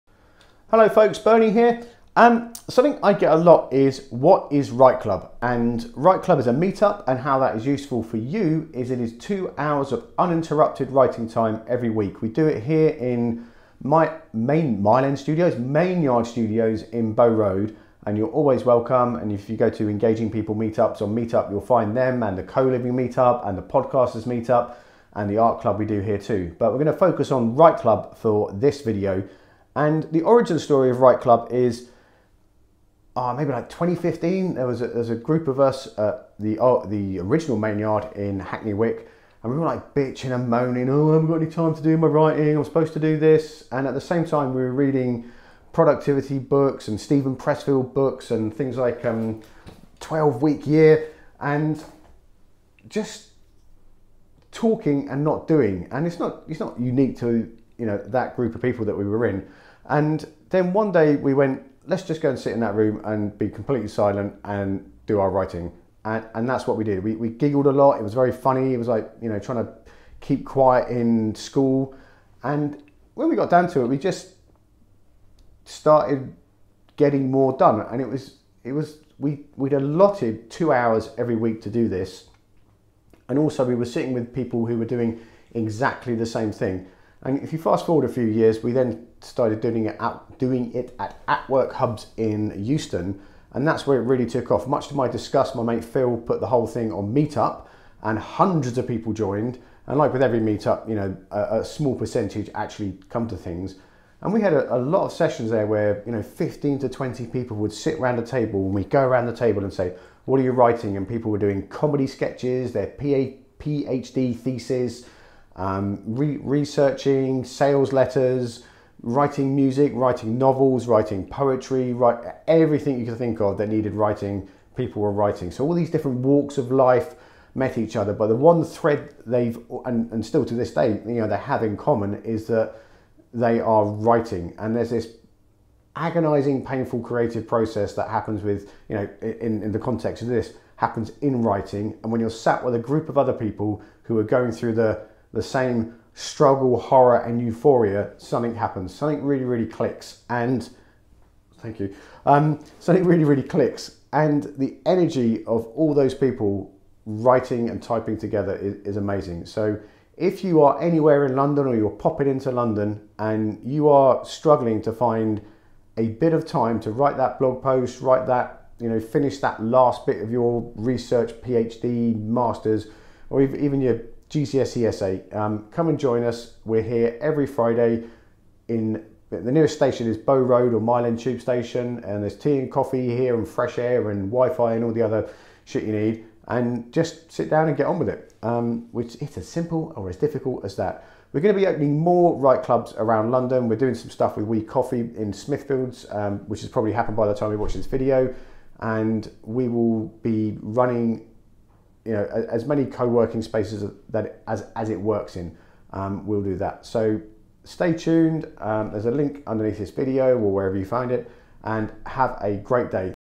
What Is Write Club Meet Up?
Every week authors, freelancers, academics, comedians, marketers, bloggers, developers, event managers, musicians, poets, lawyers and sailors gather around a table to with one goal - work on their writing for two hours.